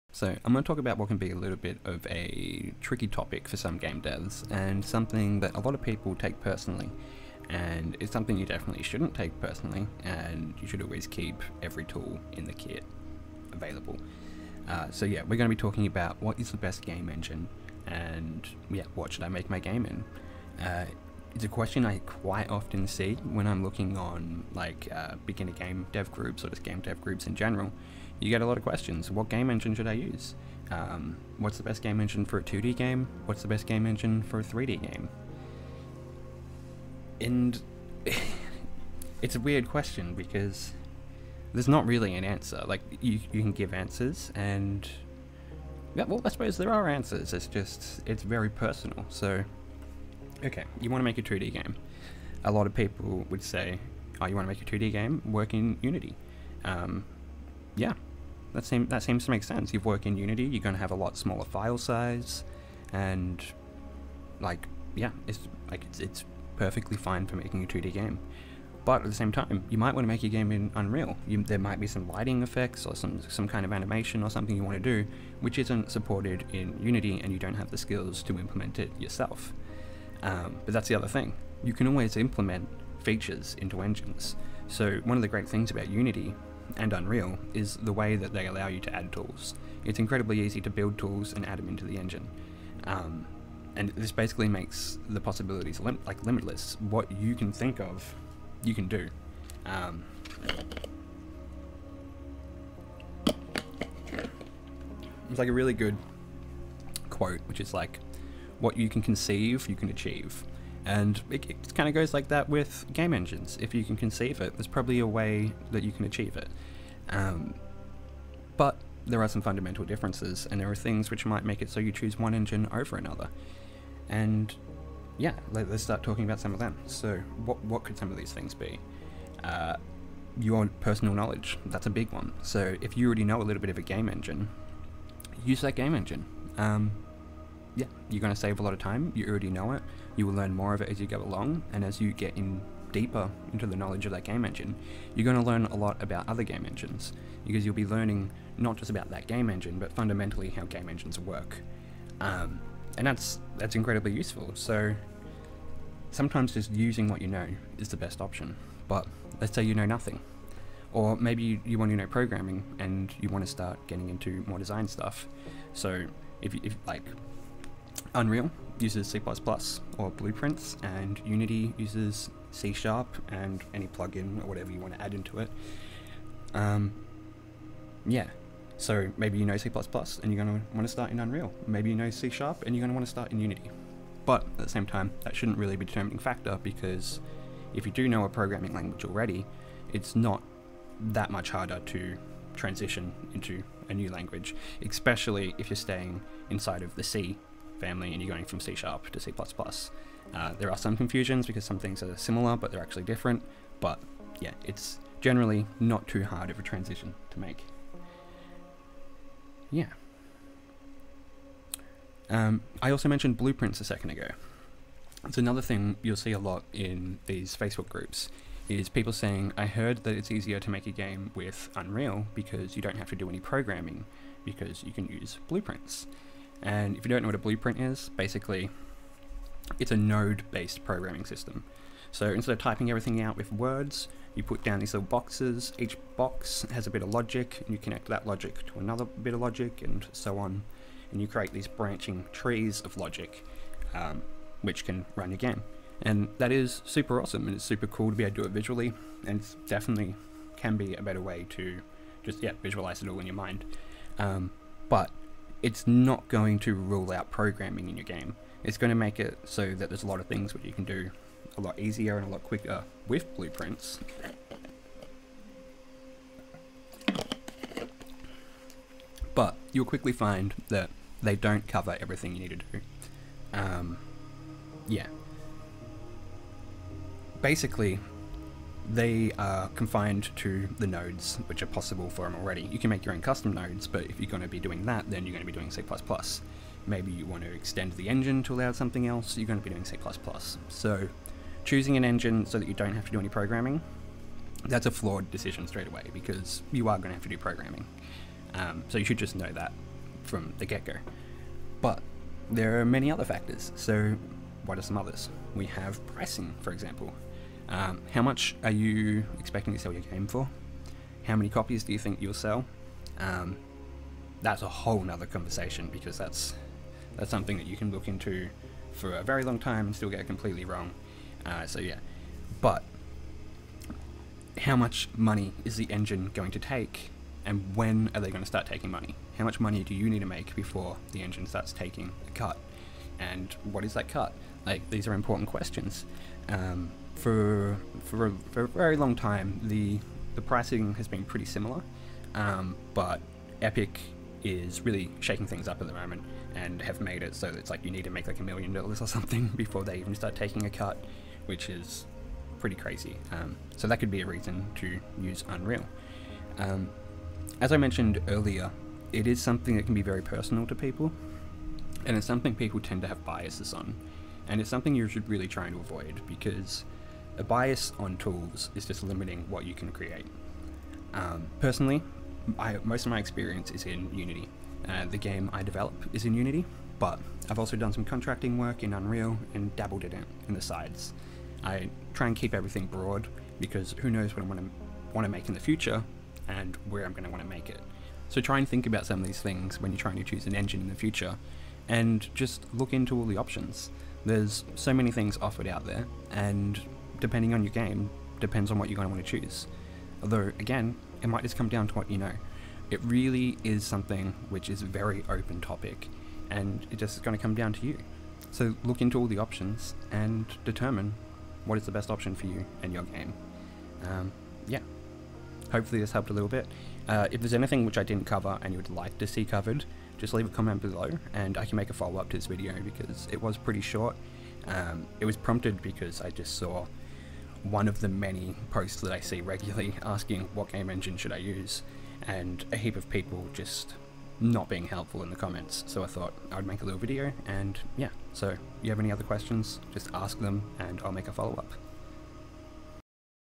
Choosing a game engine - Dev Talk - Ep 1
Welcome to this new series, Dev Talk. Where i'll discuss topics surrounding the world of game development. If you have any suggestions, drop them in the comments below or join us on Discord.